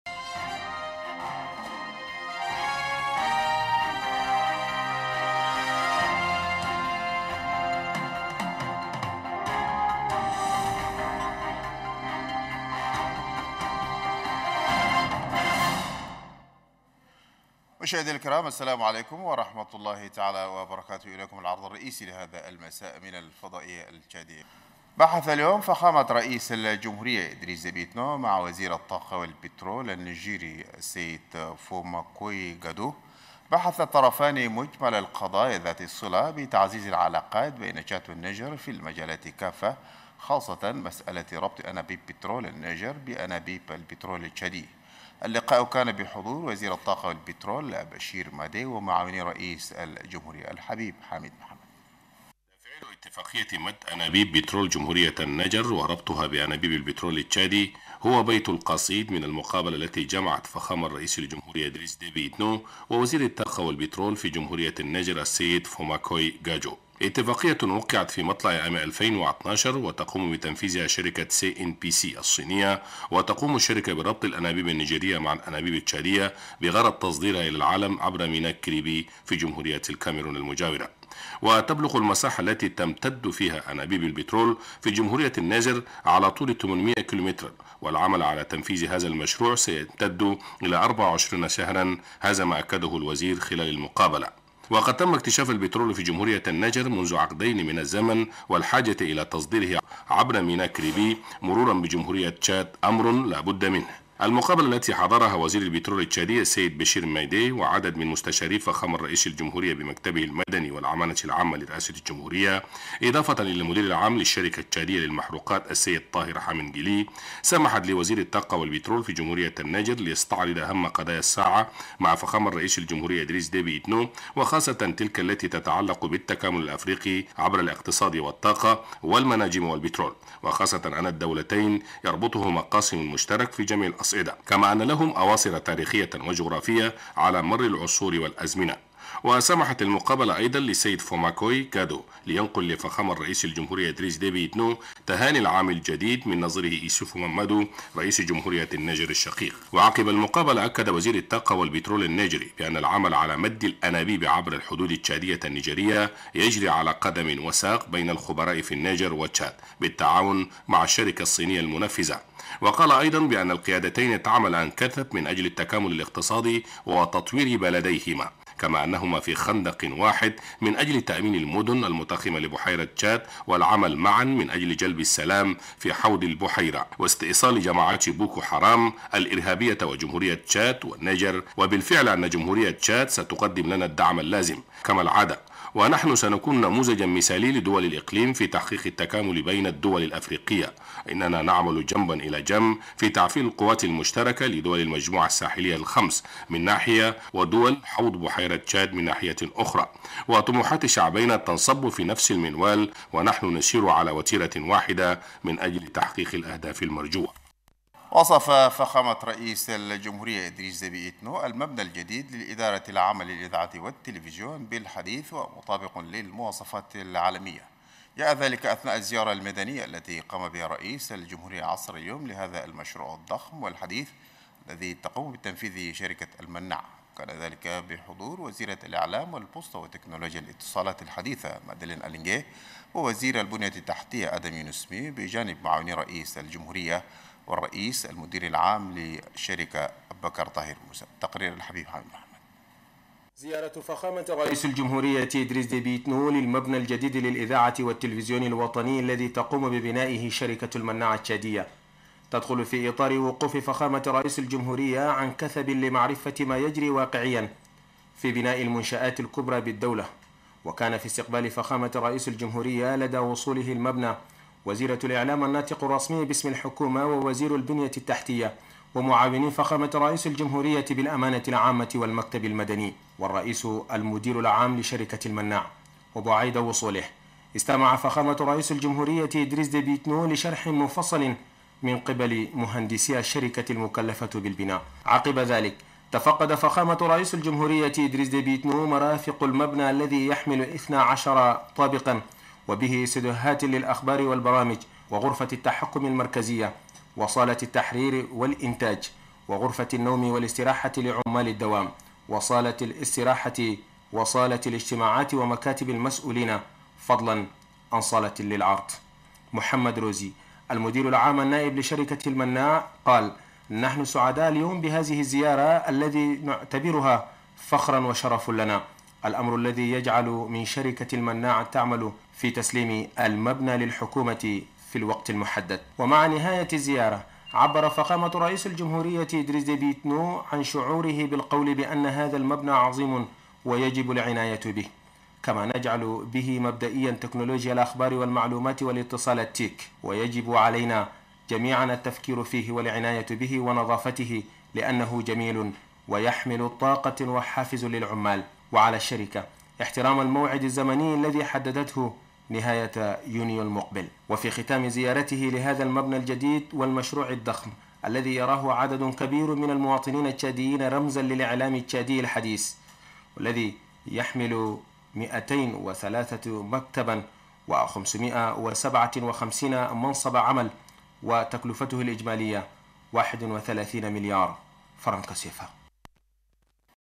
الفصل الأول من النشرة الرئيسية رئيس الجمهورية يزور المبنى الجديد للإذاعة و التلفزيون 10/01/2018
العناوين
1)  فخامة رئيس الجمهورية يستقبل وزير البترول النيجري.
2) فخامة رئيس الجمهورية يزور المبنى الجديد للإذاعة و التلفزيون.